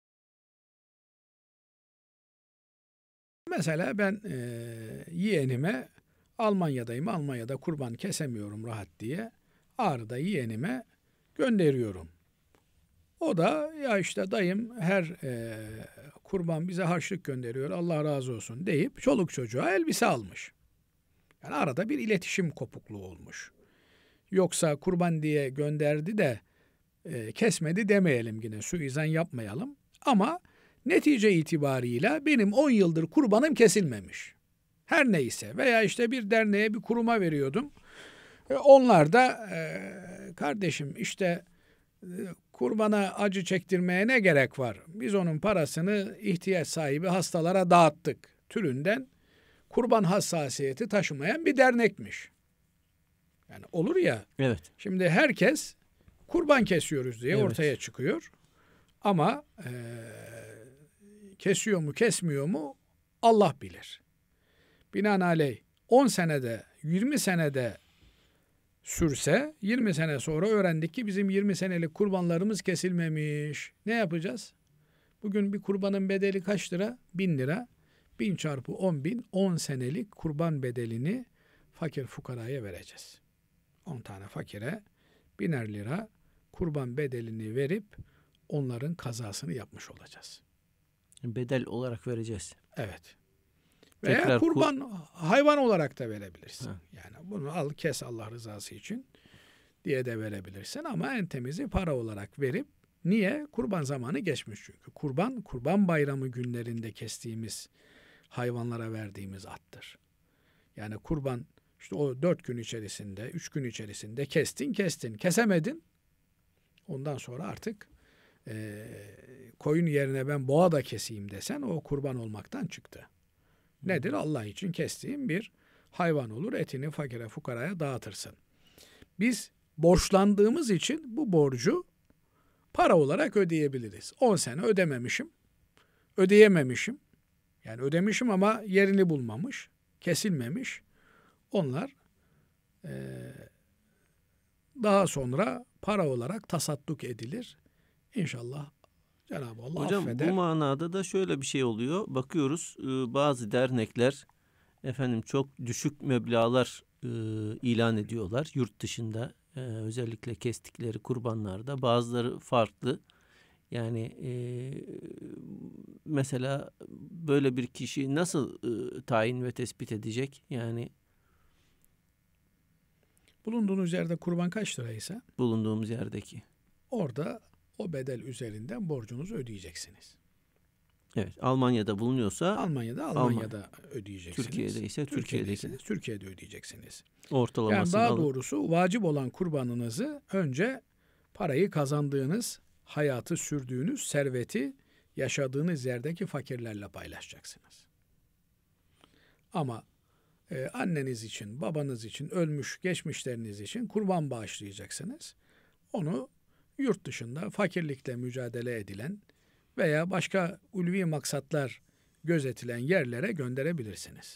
Bir Yere Kesilmesi için Vekâleten Verdiğim Kurbanım, Kesilmemiş Ne Yapmalım? - Ahmet Hamdi Yıldırım
Sorularla İslam,
Dr. Öğr. Üyesi Ahmet Hamdi YILDIRIM